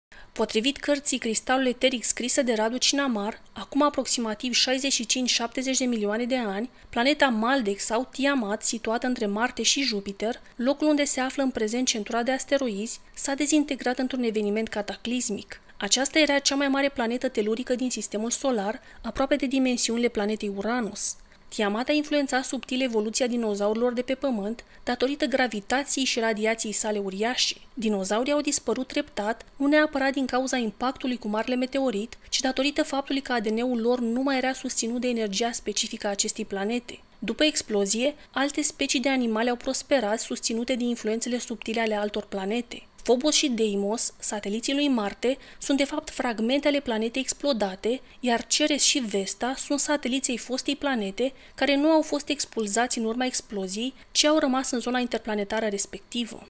Planeta TIAMAT, Centura de Asteroizi si Disparitia Dinozaurilor
sursa
cartea Cristalul eteric, autor Radu Cinamar